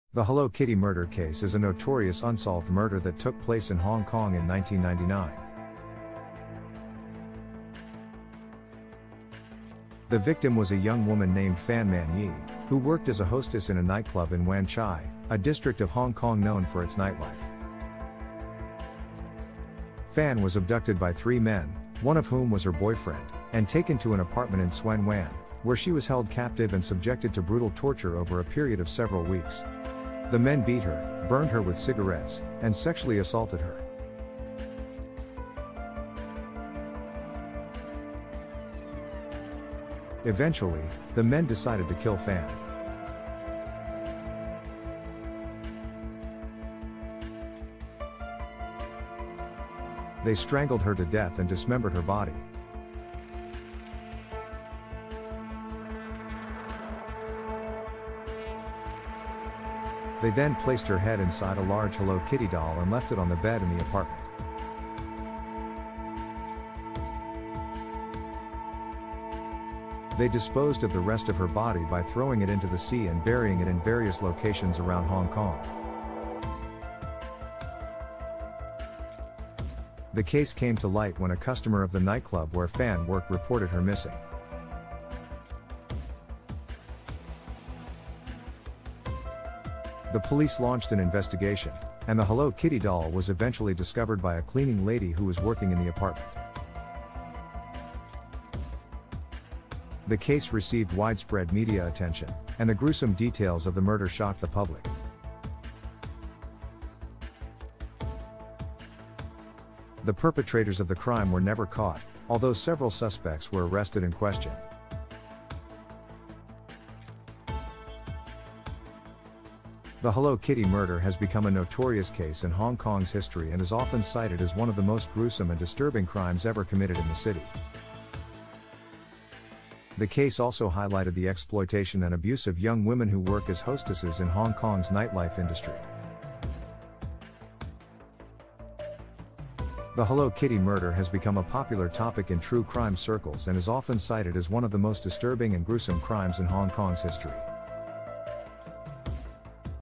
Most disturbing and Gruesome Crimes in Hong Kong "Hello Kitty Murder"
The case is known as the "Hello Kitty Murder" and is one of the most notorious murder cases in Hong Kong's history. In 1999, a young woman was found dead in an apartment in Tsuen Wan, a suburb of Hong Kong. The victim's body had been dismembered, and her head was found inside a Hello Kitty doll, which had been left on the bed.

The victim was identified as a 23-year-old nightclub hostess named Fan Man-yee. She had been abducted by three men who held her captive and subjected her to brutal torture before killing her. The men dismembered her body and disposed of the parts in various locations around Hong Kong.

The case received widespread media coverage, and the gruesome details of the murder shocked the public. Despite an extensive investigation, the perpetrators of the crime were never caught, and the case remains unsolved to this day.

The "Hello Kitty Murder" has become a popular topic in true crime circles and is often cited as one of the most disturbing and gruesome crimes in Hong Kong's history.